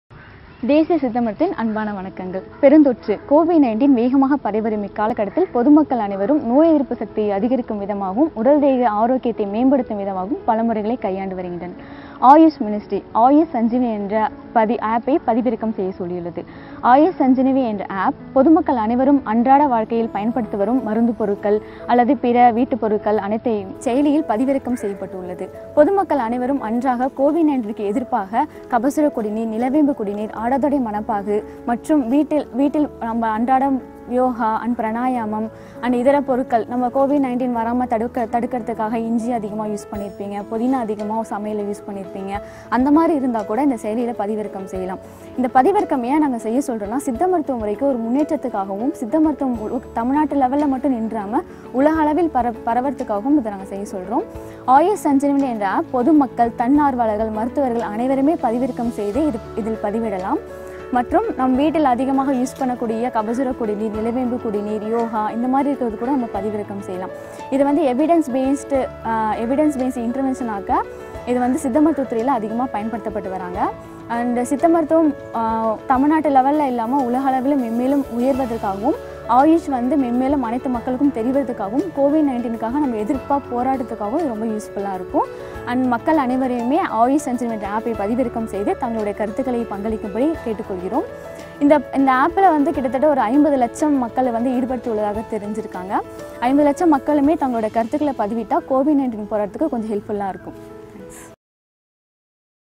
AYUSH Sanjivani Mobile Application
The Ministry of AYUSH presents the “Ayush Sanjivani” mobile application for understanding the measures adopted by public for enhancing immunity and keeping themselves healthy in the difficult COVID-19 situation.

All those who Siddha practitioners/General public/ volunteer/ to furnish information through this app will be making a significant contribution to the study of Siddha public health in the country.

I recommend Ayush Sanjivani app to give feedback for immunity boosting measures against COVID-19.

NIS requested to kindly download AYUSH Sanjivani mobile app and upload to experience in Siddha medicine in this COVID19 pandemic and support us for big data analysis.